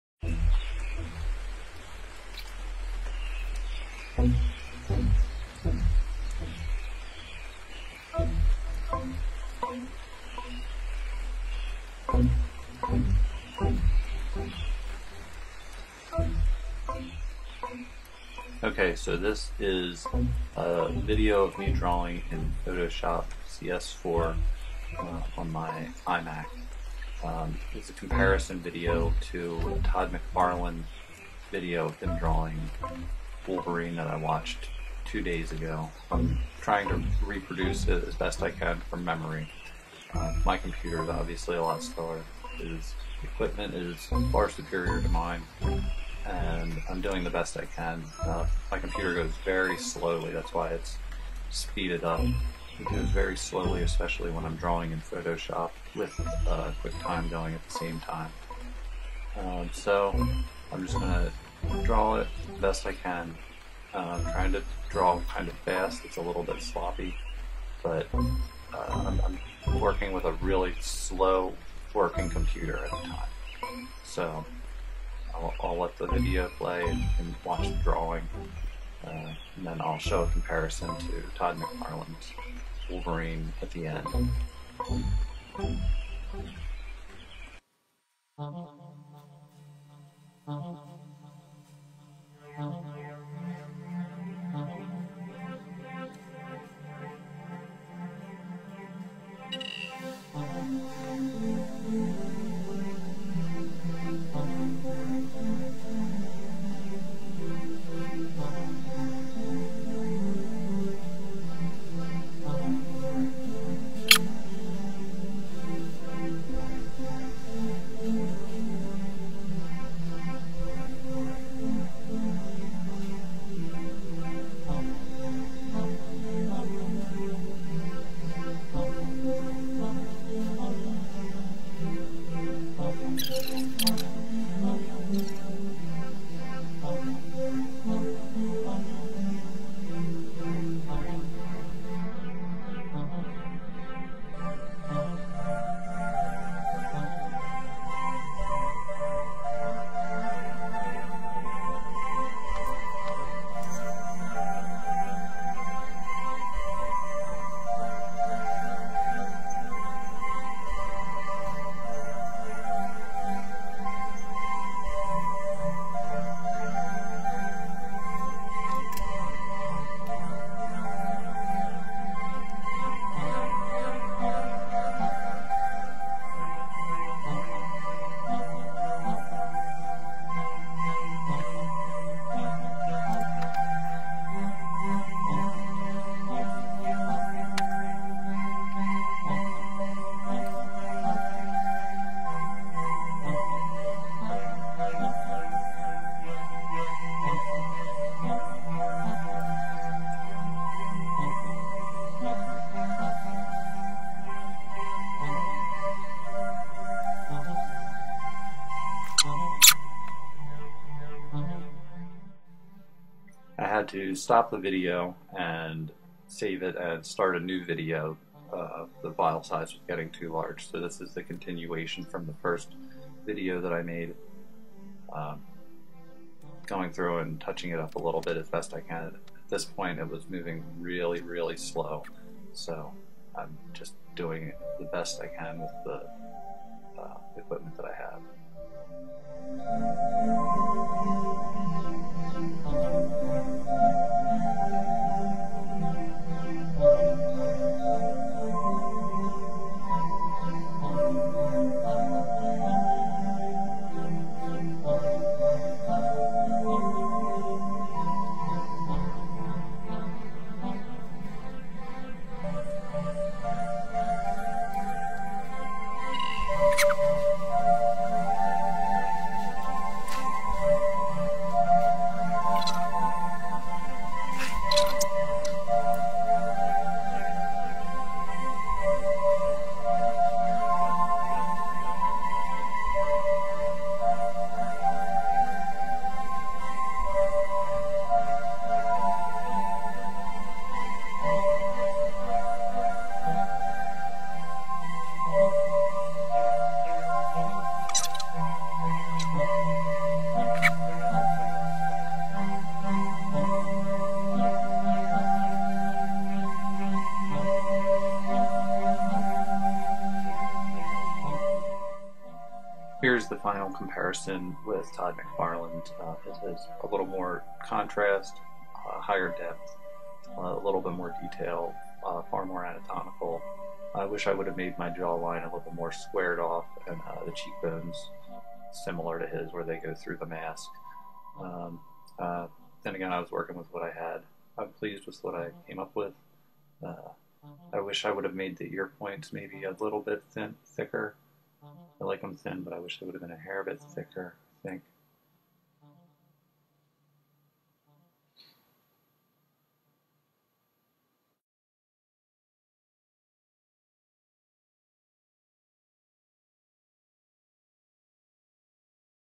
Drawing Wolverine Photoshop CS4 Todd McFarlane Comparison
Time Lapse Drawing of Wolverine in Photoshop CS4 with a Wacom Bamboo Pen and Touch. Inspired By Todd Mcfarlane Drawing (Speed Drawing) (Drawing From Memory)